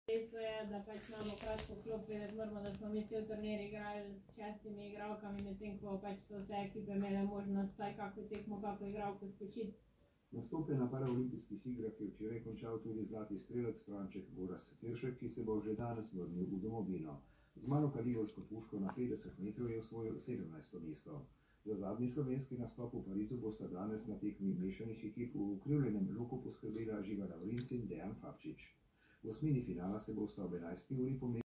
2024-09-05 LJUBLJANA Doma CARNIVORA Felis catus DOMAČA MAČKA (Bunkica: dobro jutro, dober tek ...)
... kaj, a ni nič pravega za jest? ...